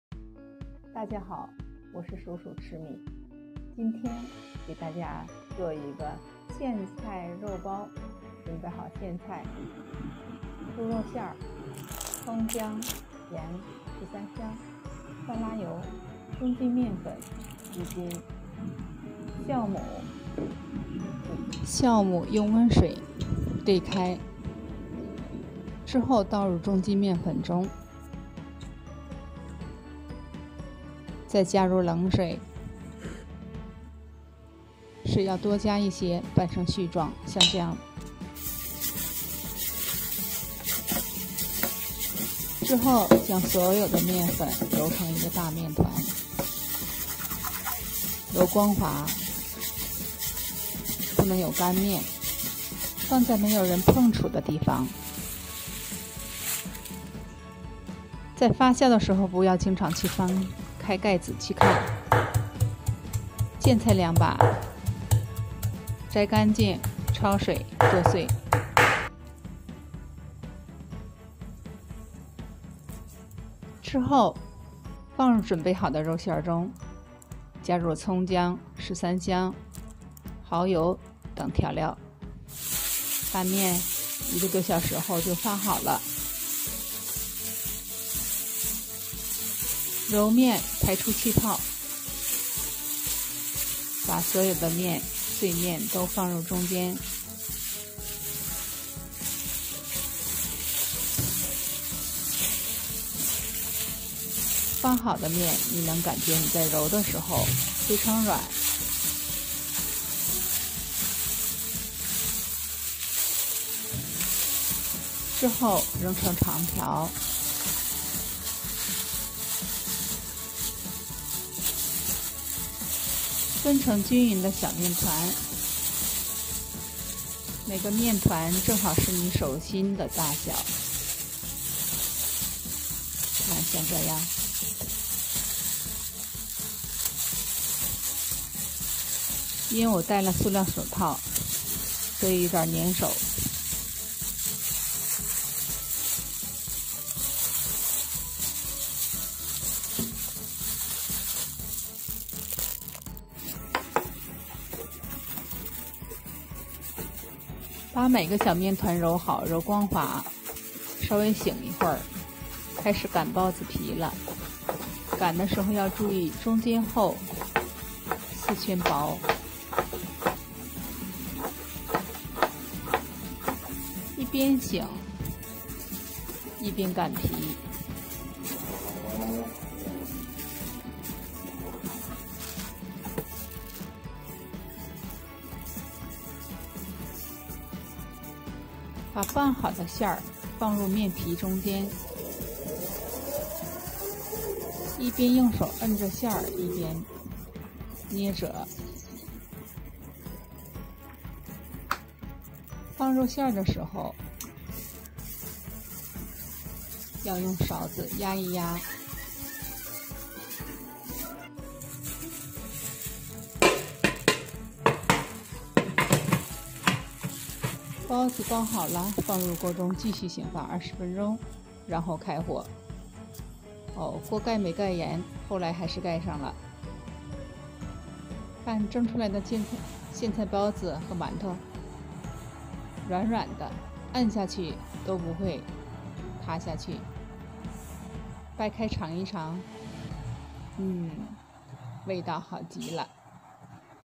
苋菜肉包，苋菜包子你没吃过！快来看看怎么做!
准备材料：小苋菜两小捆、大葱半根、猪肉馅儿半斤、姜末、眼、花椒水、十三香粉、蚝油、
色拉油、中筋面粉一斤、发酵粉5克

将发酵粉用温水调匀，将一斤的中筋面粉和发酵粉活在一起，
揉到光滑。放到别人碰不到并且温暖的地方，不要一直掀开看
，大概一个小时就发好了。发面期间把苋菜一根一根扒皮洗干净。
之后放入水中焯一下，捞出切碎。把肉馅放入盐、蚝油、
花椒水、十三香调好
备用。